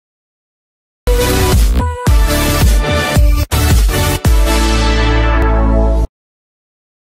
The Calling part 17
My first ever MAP part! Thank you to the host for letting me join.

Time taken: Around 6 hours
Script: Longtail’s defeated, Rusty looks up at Bluestar. Bluestar smiles down at him.
Characters: Rusty, Longtail, Bluestar
Finished Map:

Programs(They are all free):
Firealpaca for drawing
Hitfilm Express for editing and tweening